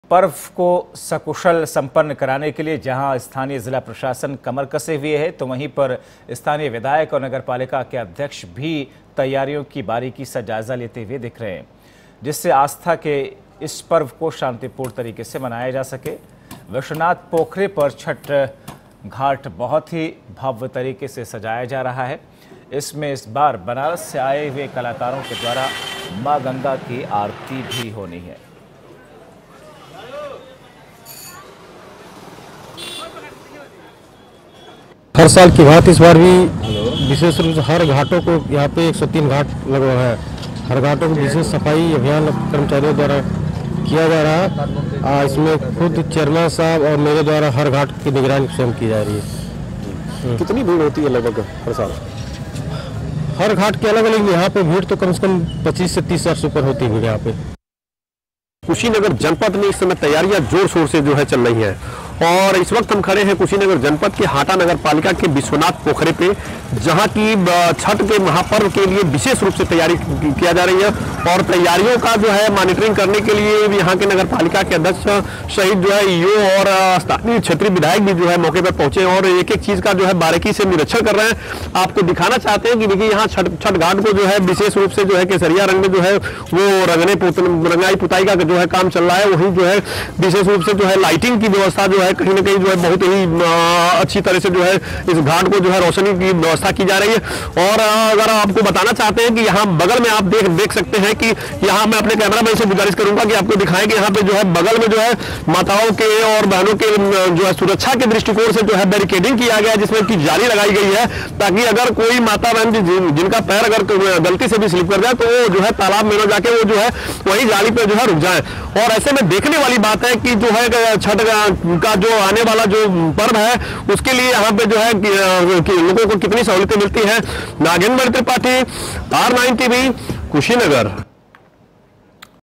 UP: कुशीनगर में छठ पर्व सकुशल मनाने की तैयारी
कुशीनगर में छठ पर्व सकुशल मनाने की तैयारी..घाटों की हो रही है सफाई..छठ घाट को भव्य तरीके से सजाया गया